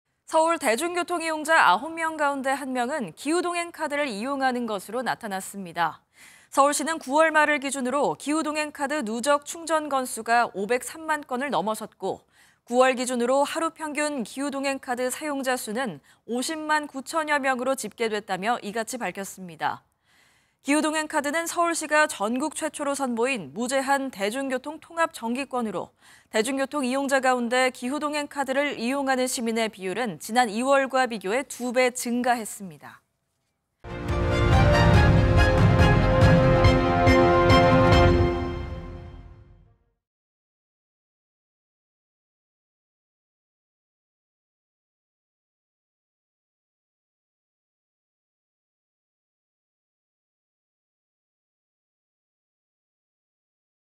서울 대중교통 이용자 9명 중 1명 기후동행카드 사용 [전국네트워크]
서울 대중교통 이용자 9명 가운데 1명은 기후동행카드를 이용하는 것으로 나타났습니다.

  서울시는 9월 말을 기준으로 기후동행카드 누적 충전 건수가 503만 건을 넘어섰고 9월 기준으로 하루 평균 기후동행카드 사용자 수는 50만 9천여 명으로 집계됐다며 이같이 밝혔습니다.

  기후동행카드는 서울시가 전국 최초로 선보인 무제한 대중교통 통합 정기권으로, 대중교통 이용자 가운데 기후동행카드를 이용하는 시민의 비율은 지난 2월과 비교해 2배 증가했습니다.